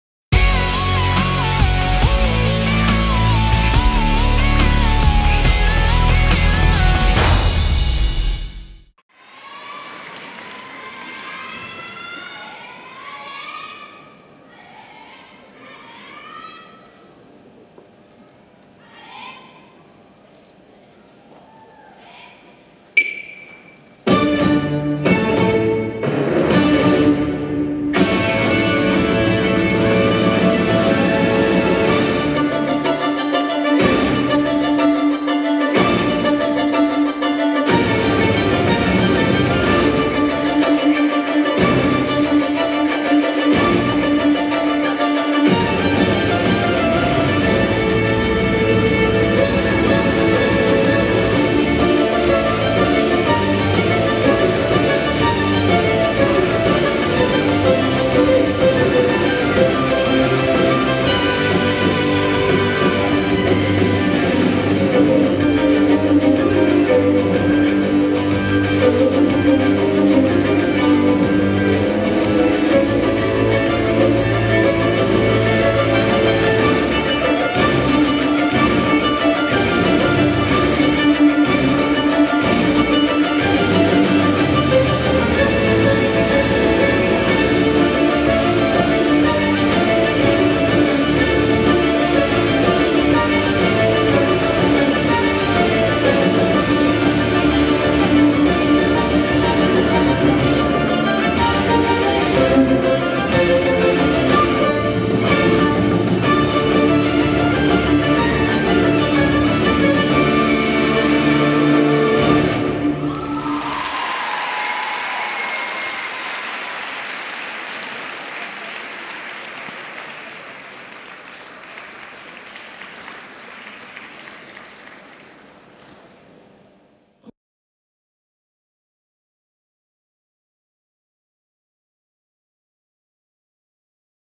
Thiais 2015 Senior 13 Katsiaryna HALKINA BLR
Grand-Prix Thiais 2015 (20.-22. March)
Rhythmic Gymnastics Grand-Prix in Thiais / France